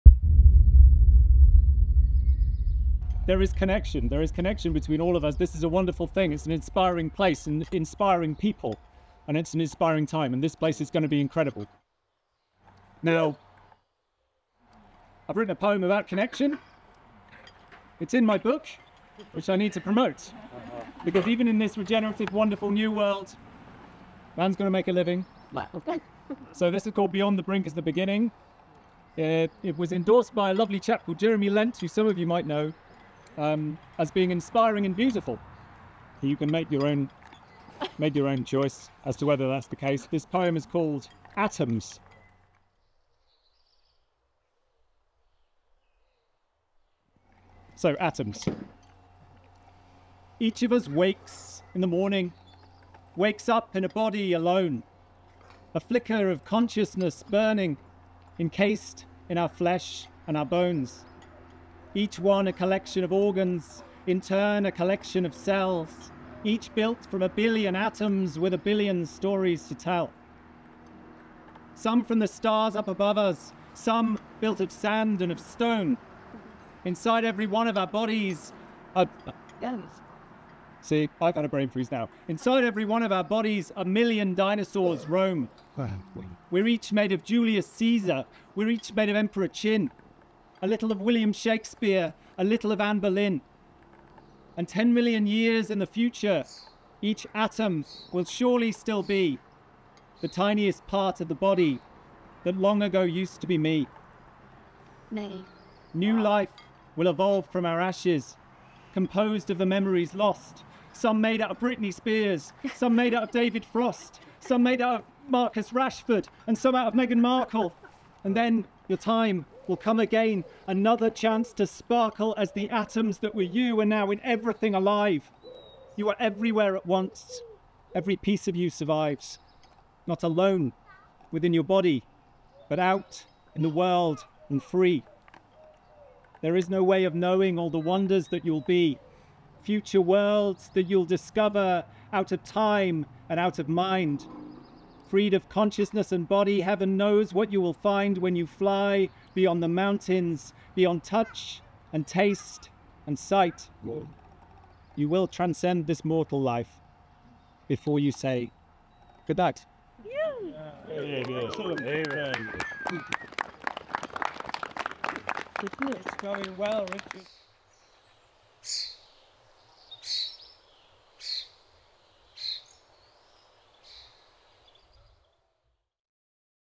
Richard Wain reads 'Atom' a poem from his new book Beyond the Brink is the Beginning
Richard Wain and Marcus Link (Our Co CEO and Co Founder) once ran a business together and remain very great friends. Richard offered to read a poem at our Inauguration Ceremony on 20th June 2024. The poem is titled Atom and is from his new book Beyond the Brink is the Beginning. available now.